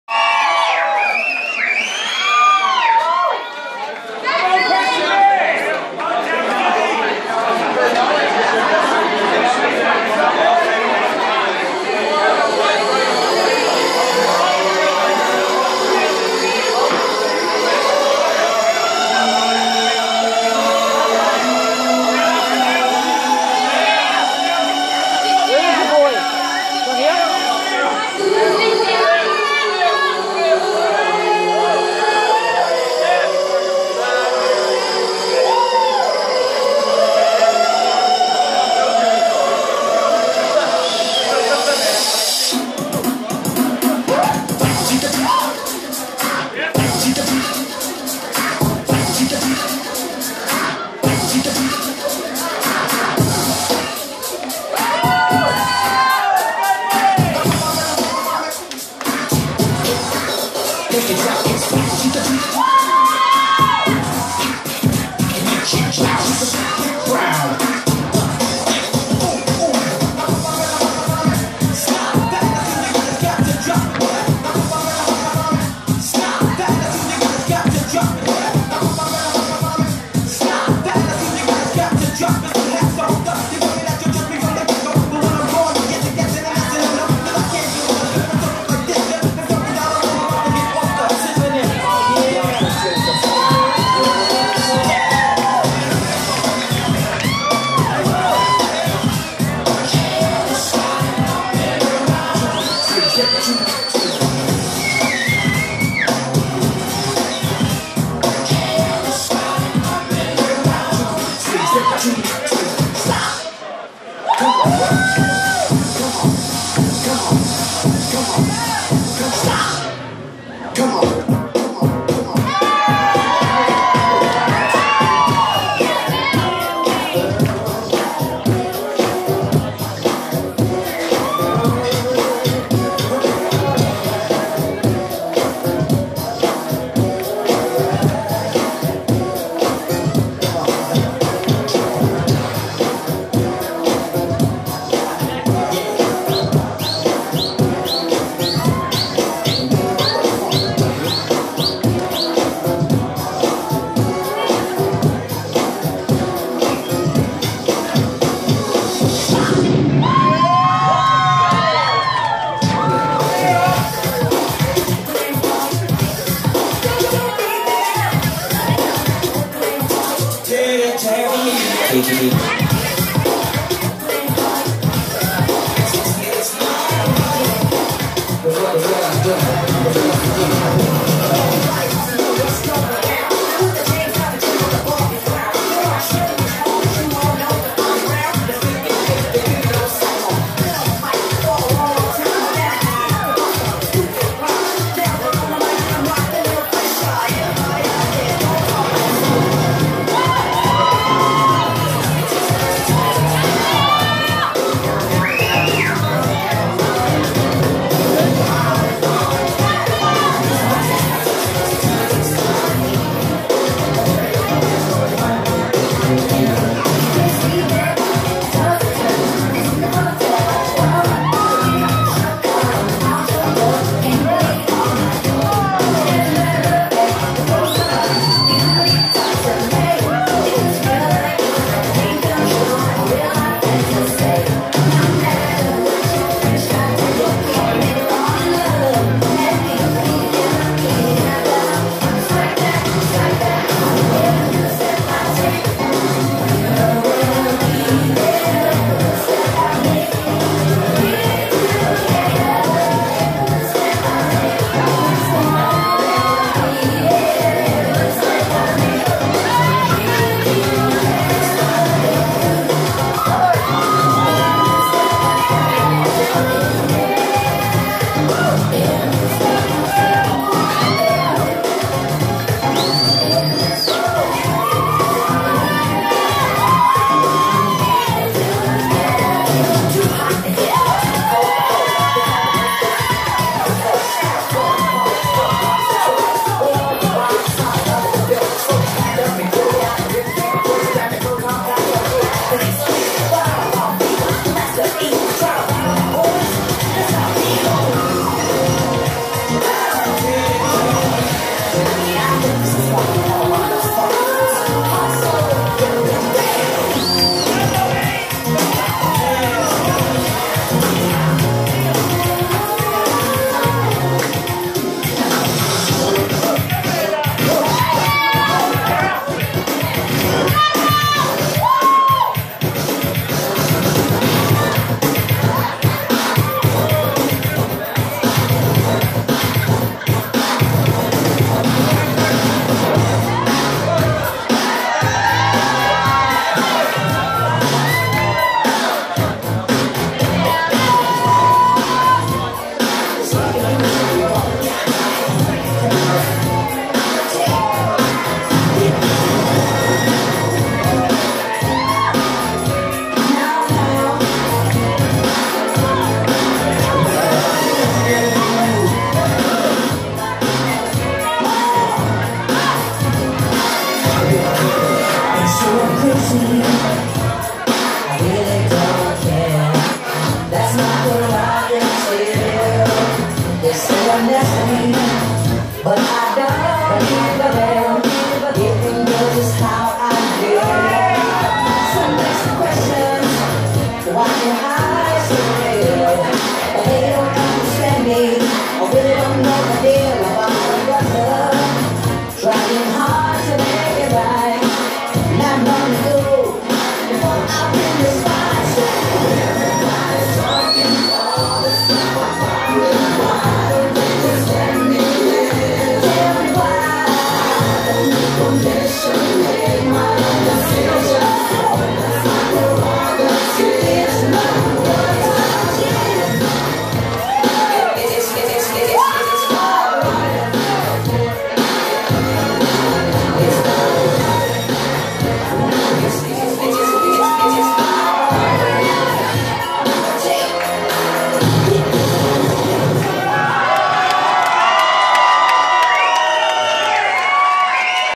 New Jack Swing/Bobby Brown Dance Show ft Phil Haddad.
Melbourne New Jack Swing Bobby Brown Dance Tribute with well known dance master Phil Haddad & crew getting down.